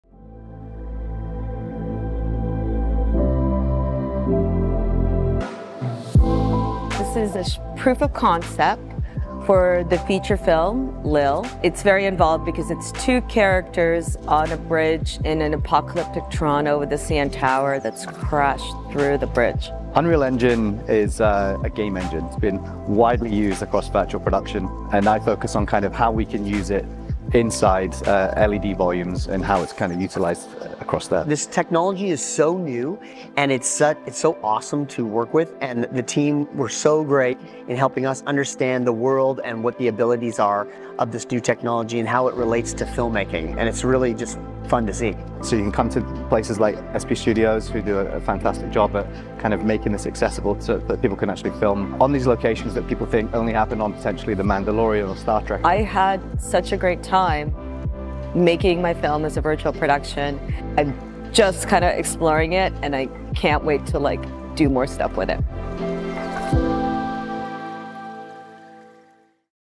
SP Studios: Behind the Scenes "Lil" - Making a Post-Apocalypse Toronto
Behind the Scenes of "Lil" - a virtual production short film shot at SP Studios.

Director: Virginia Abramovich
Producer(s): Alex Jordan, Virginia Abramovich
Production Company: A Jordan Entertainment & Artificial Dreams Production
Director of Photography: Nelson Rogers
Production Designer: Laura Keightley
Unreal Engine Specialist: Parth Bhagat
Virtual Production Supervisor: Conor McNeillly
Studio Manager: Adam Shone